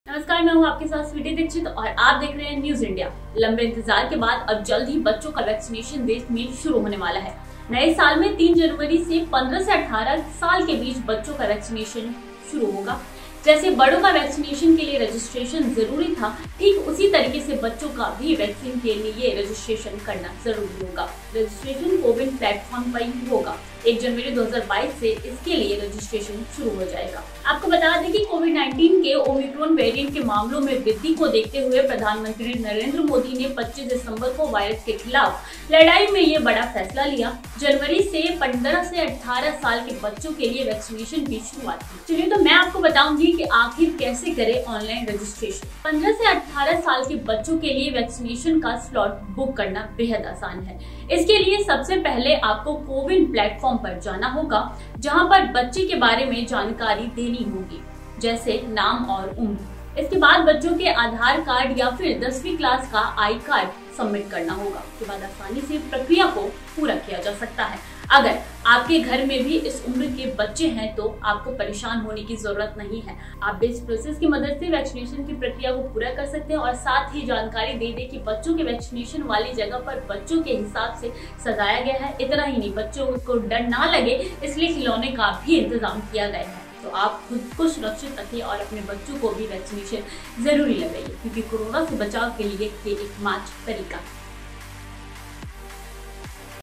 Child Vaccination देश में शुरू होने वाला है जानिए registration की प्रक्रिया | Omicron India
बड़ों को वैक्सीशन के लिए रजिस्ट्रेशन जरूरी था ठीक उसी तरीके से बच्चों को भी  वैक्सीन लगवाने के लिए रजिस्ट्रेशन करवाना होगा बच्चों का रजिस्ट्रेशन भी कोविन प्लेटफोर्म पर होगा 1 जनवरी 2022 से इसके लिए रजिस्ट्रेशन शुरू हो जाएगा